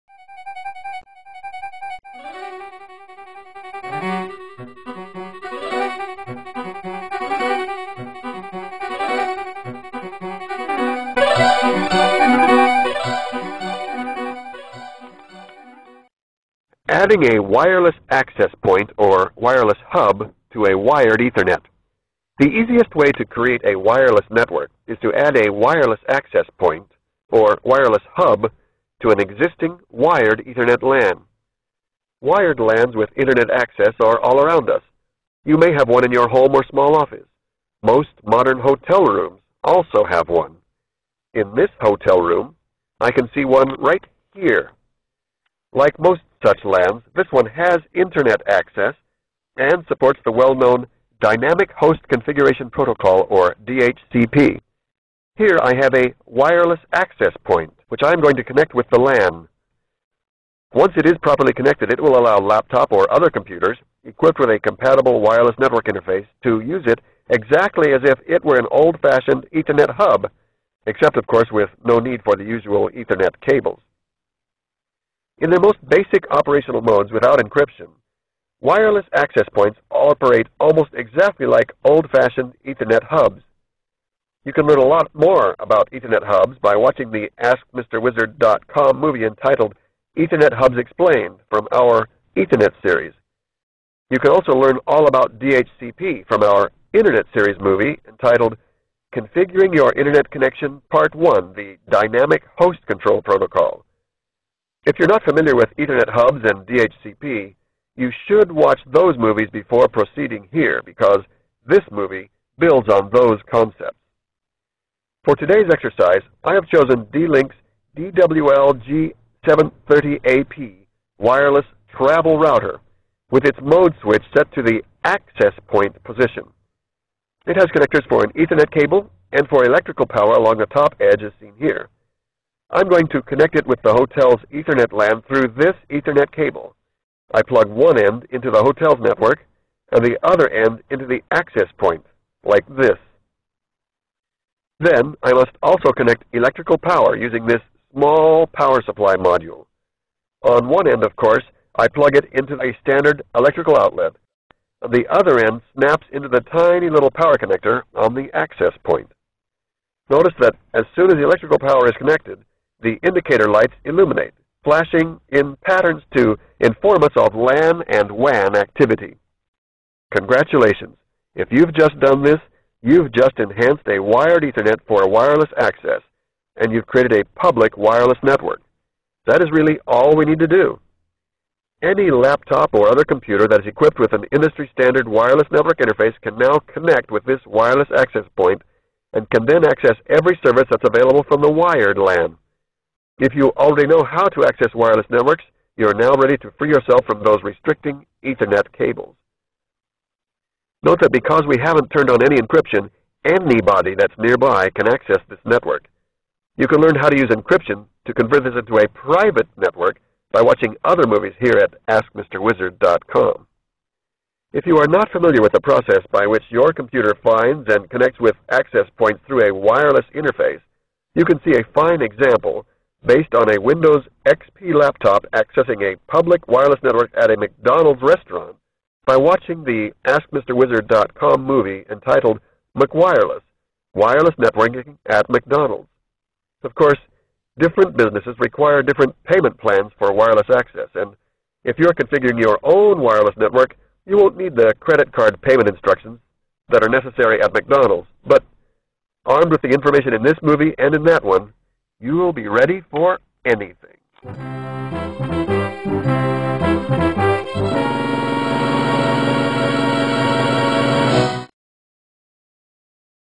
Easiest Wireless Lan AskMisterWizard
AskMisterWizard.com Networking Fundamentals Section: Adding a "Wireless Access Point" to a wired Ethernet LAN

The easiest way to create a wireless network is to add a "Wireless Access Point" to an existing, wired Ethernet that already has Internet access and supports the "DHCP" protocol for automatic configuration. This 5-minute movie shows exactly how I did it in a hotel room, using a low-cost "Travel Router" in "Wireless Access Point" mode.